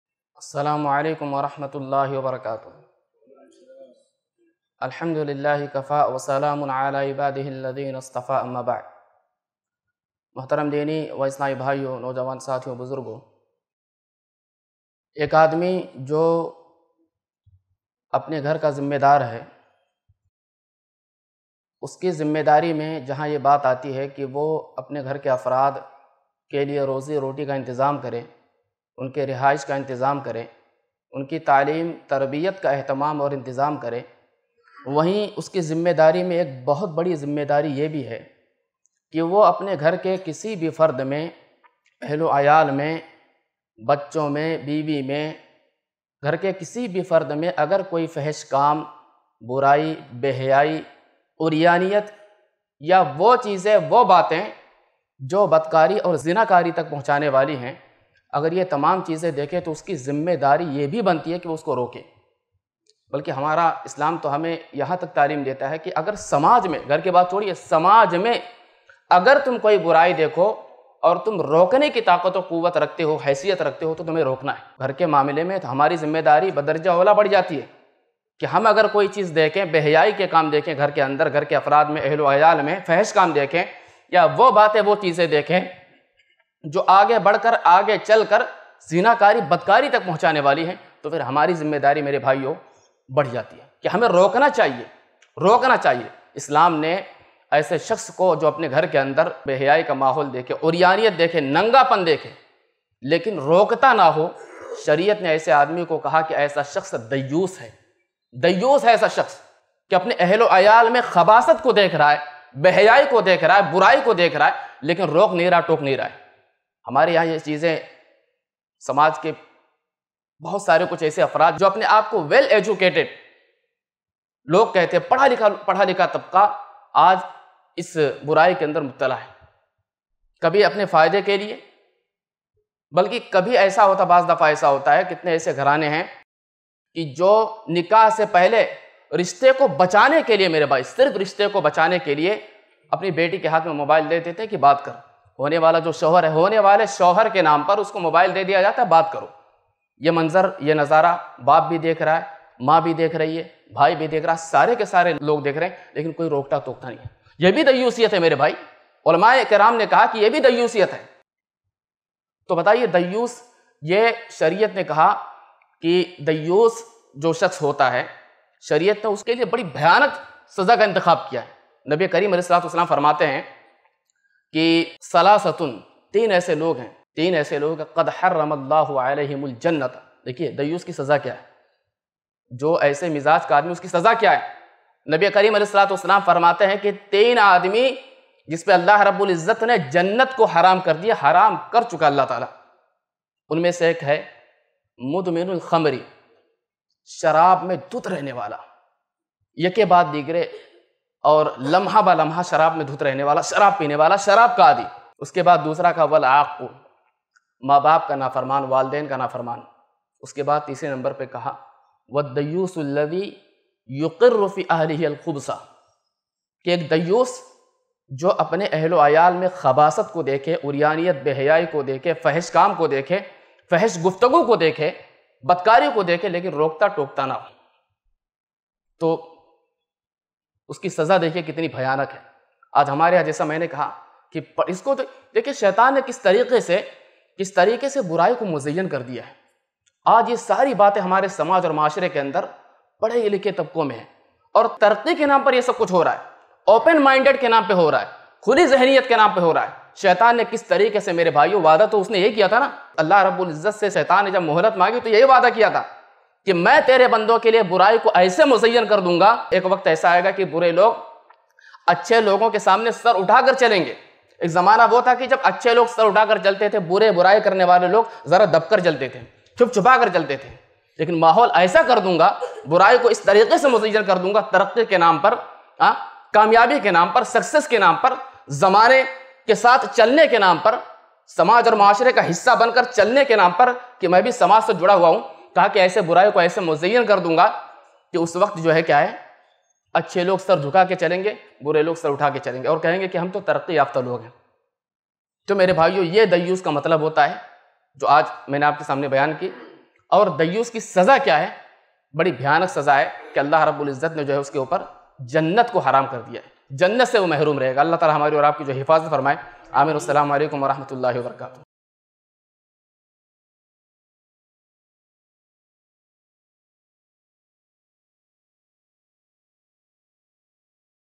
Dayyus Kise Kehte Hain ? Dayyus Kaun Hai Aur Uski Saza Kya Hai? Asaan Deen Ep. 67 | IIC Mumbai
Bank Account Details:
ILM FOUNDATION
ICICI Bank
Savings account
IFSC CODE-ICIC0001028

Dayyus Kise Kehte Hain ? Dayyus Kaun Hai Aur Uski Saza Kya Hai By Shaikh Fayyaz Mustaqueem Muhammadi | Asaan Deen Ep. 67 | IIC Mumbai